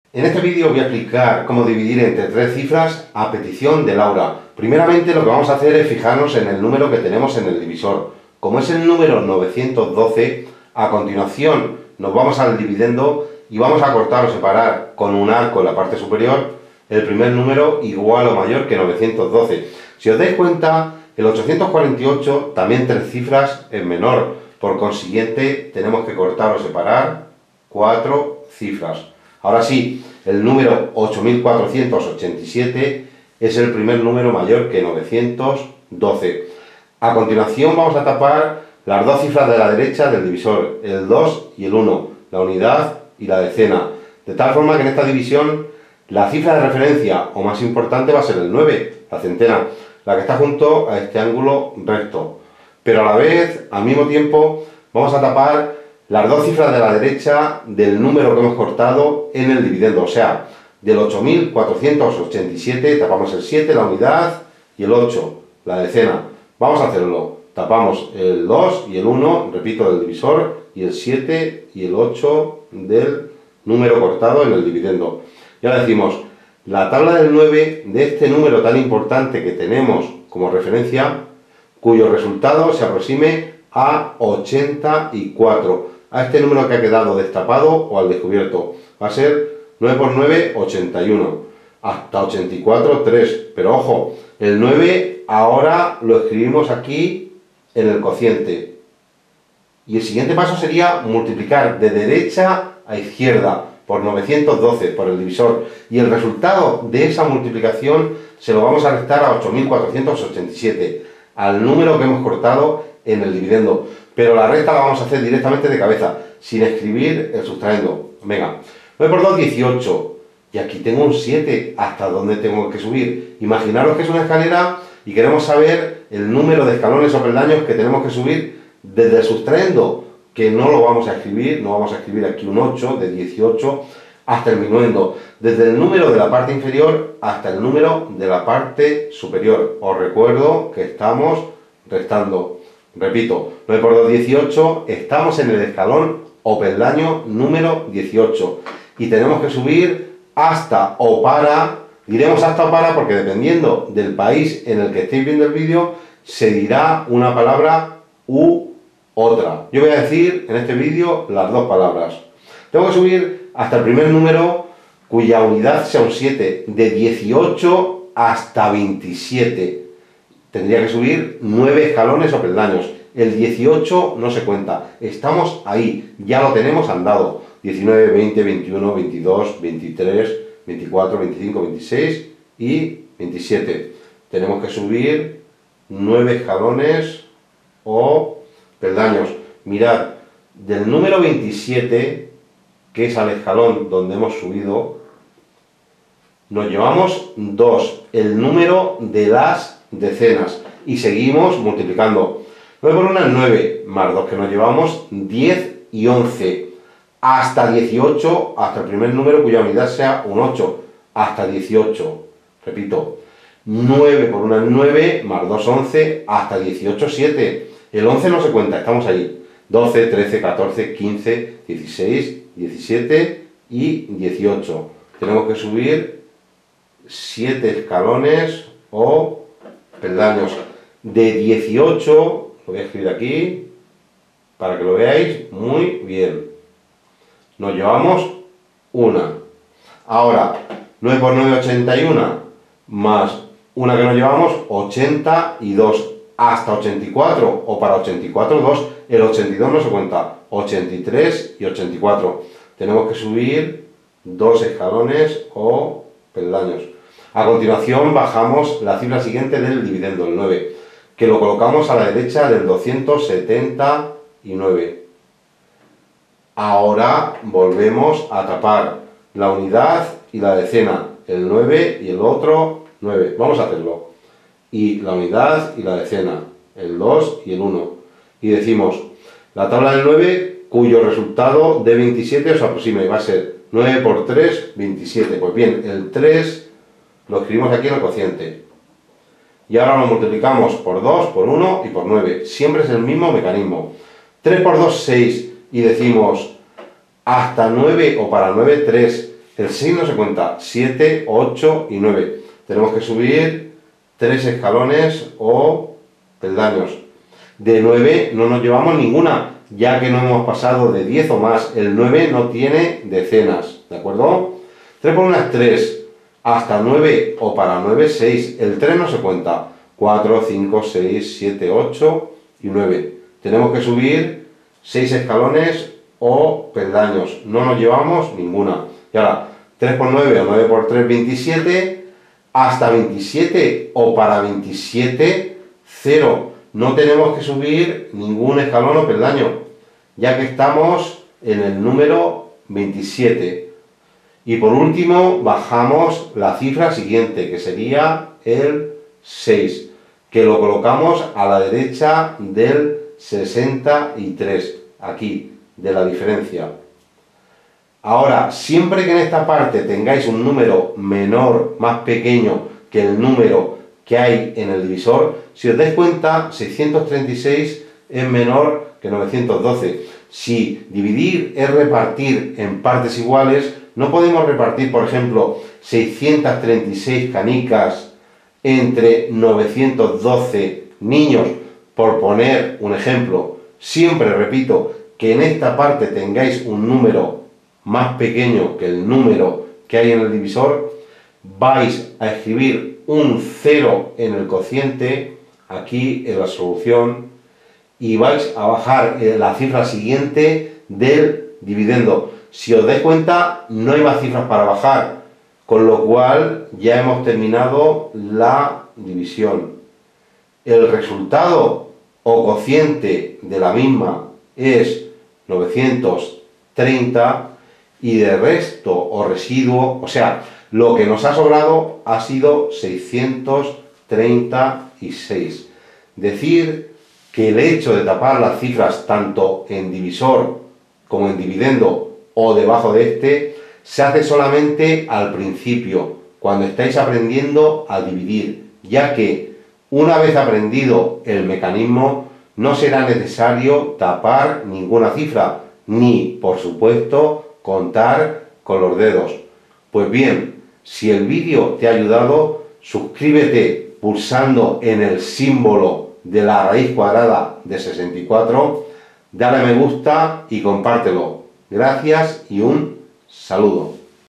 Enseñar A UN NIÑO a dividir por 3 cifras
Os explico cómo dividir por 3 cifras